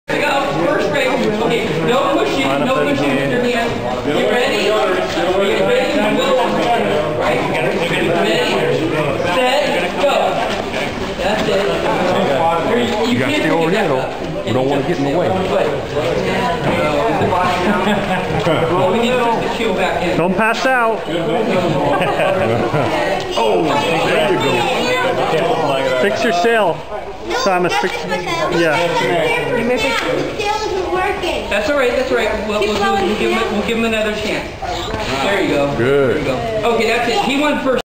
T's boat race (HQ)
T races his balsa wood sail boat in the Rain Gutter Regatta at the spring cub scout campout.  4/07